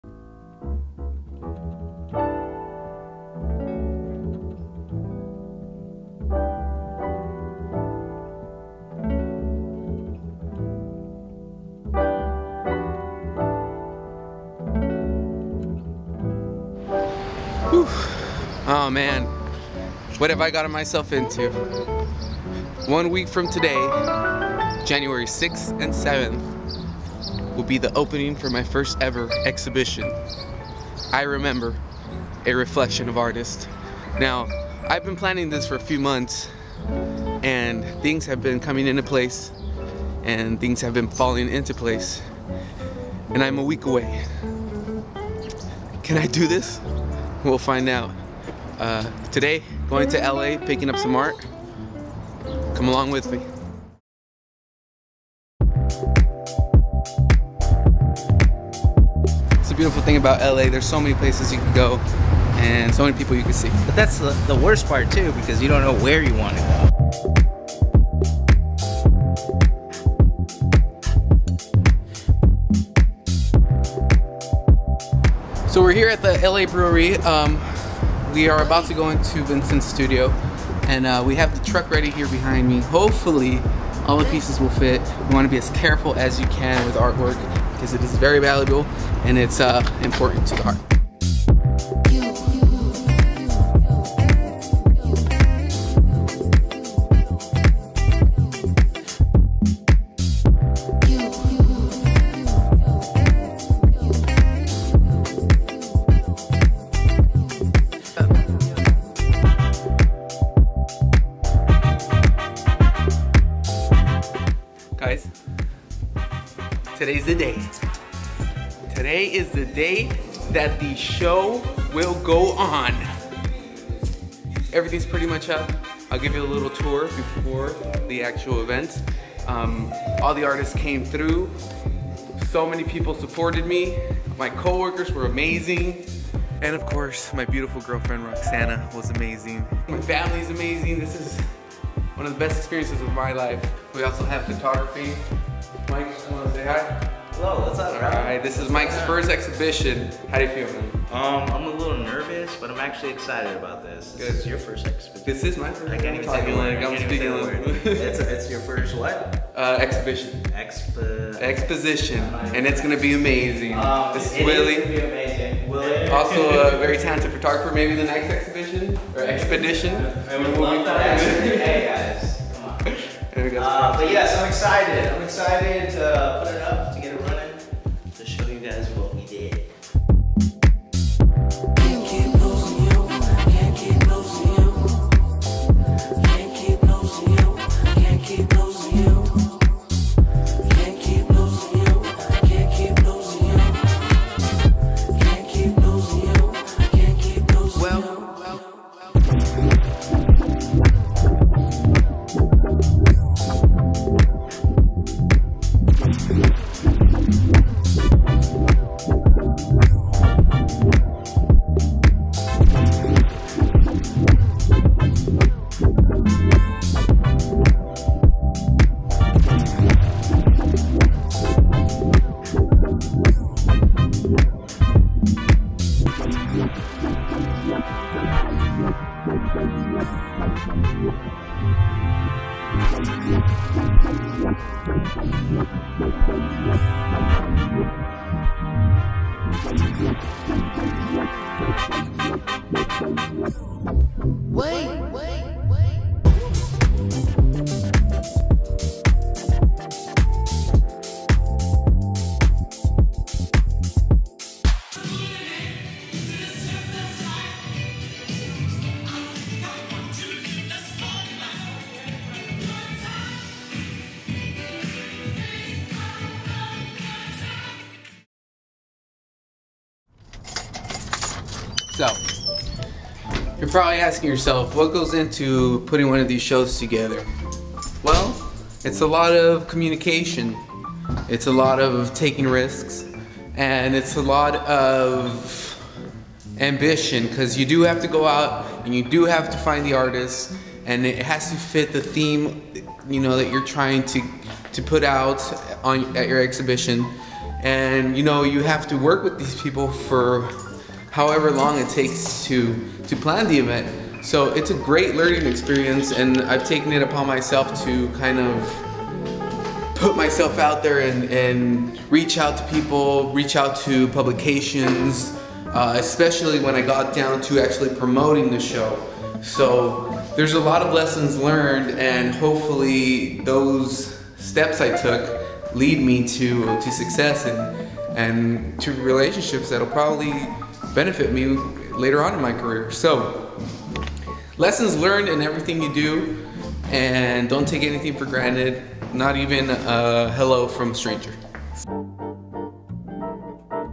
inTORRESting Ep 20 I Remember Exhibition
January 6th, I accomplished something I never thought I would. I curated my very first exhibition. I Remember: A Reflection of Artists was something I only imagined. An Idea. A dream that became a reality. The following footage is proof of those events. Enjoy!